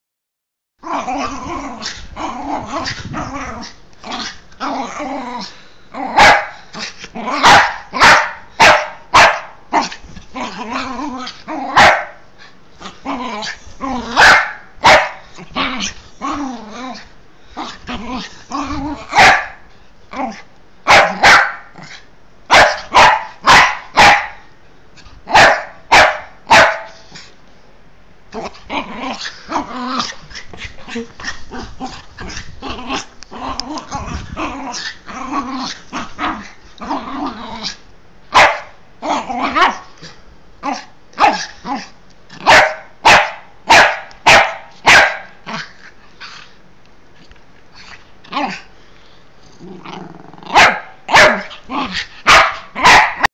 Hyper Dachsund.
Howard has 1 too many cups of coffee.